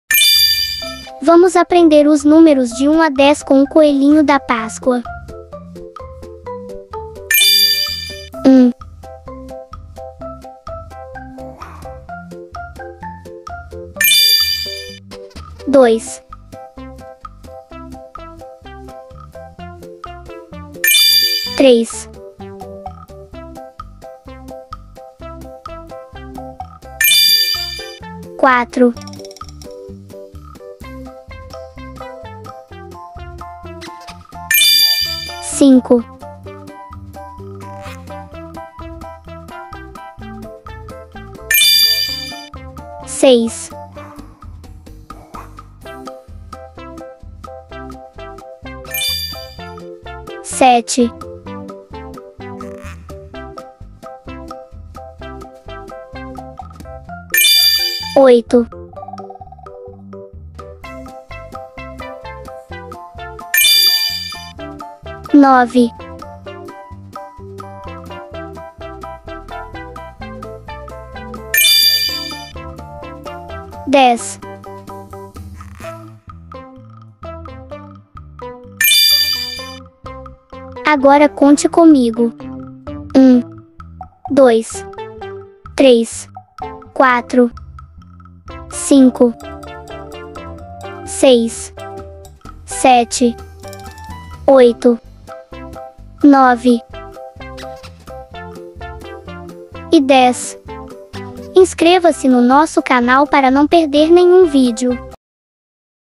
Vamos aprender os números de 1 a 10 com o coelhinho da páscoa.
Vamos contar os ovinhos do coelhinho da páscoa e aprender os números de 1 a 10.

Canal Infantil Lukinhas e Davi

Vamos brincar e aprender com muita diversão.